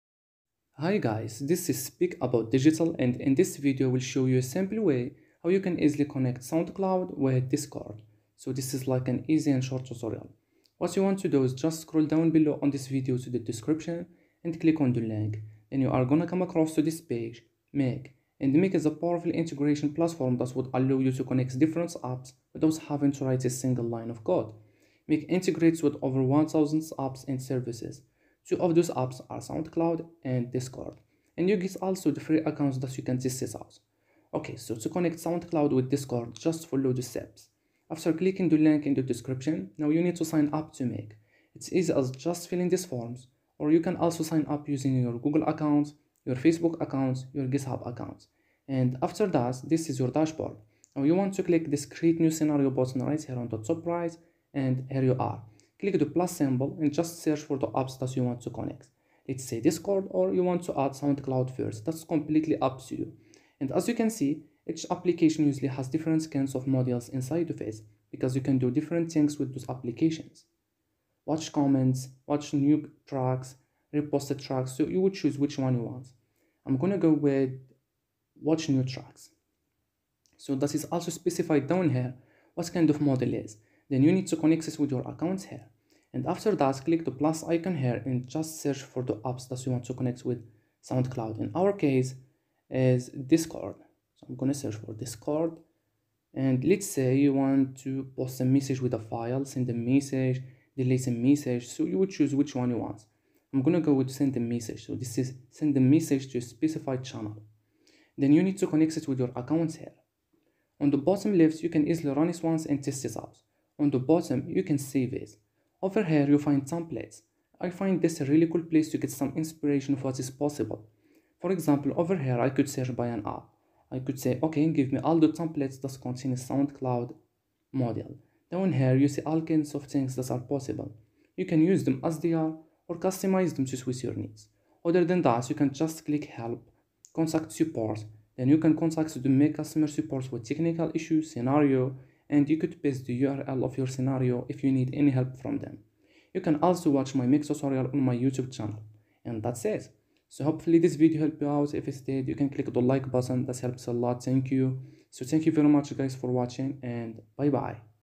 How to Connect SoundCloud to Discord
In this video you will find out How To Integrate SoundCloud with Discord

🔥 Special Offers 🔥